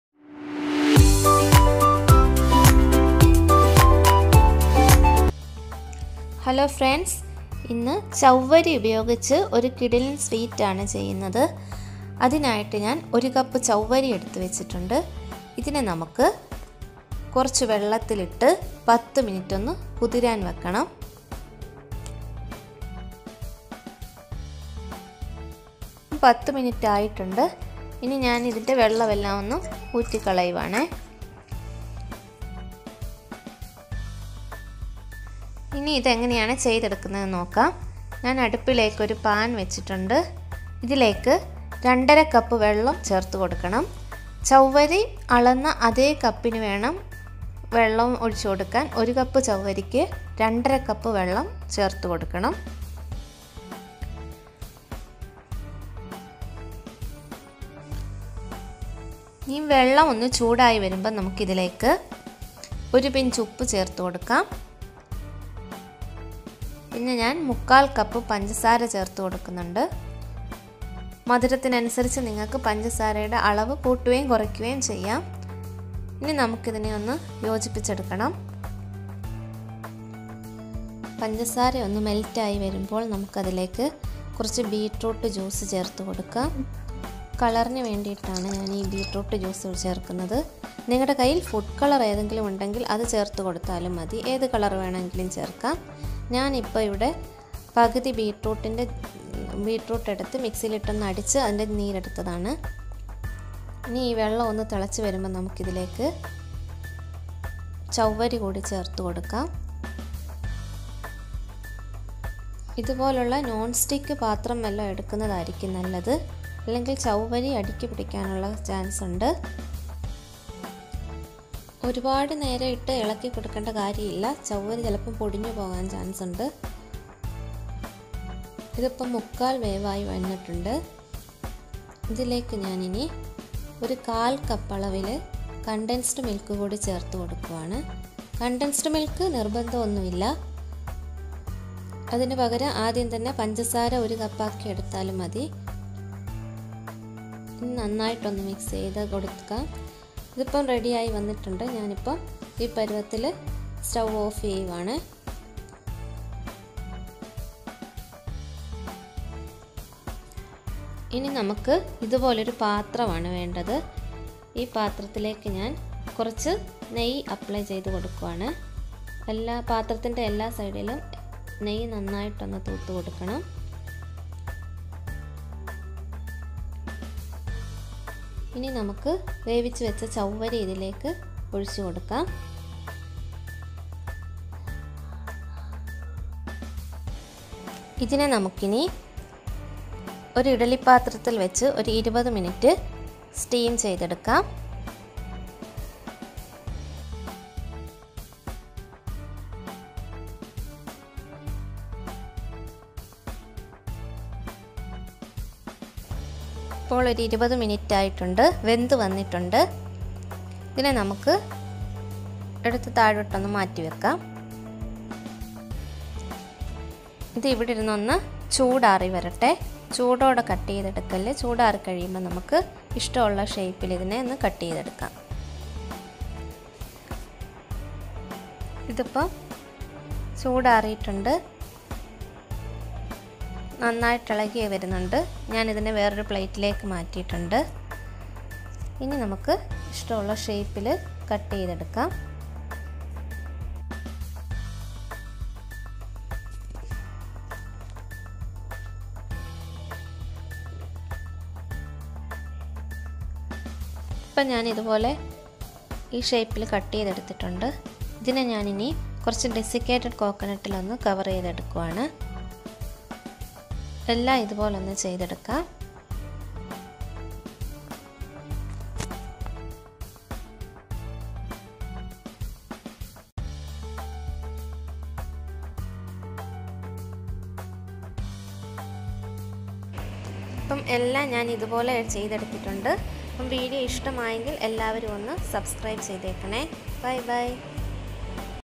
Sabudana Sweets// ചവ്വരി സവീററ്സ്
INGREDIENTS

Sabudhana(sago)
Water
Beetroot juice
Sugar
Condensed milk
Descicated coconut
Salt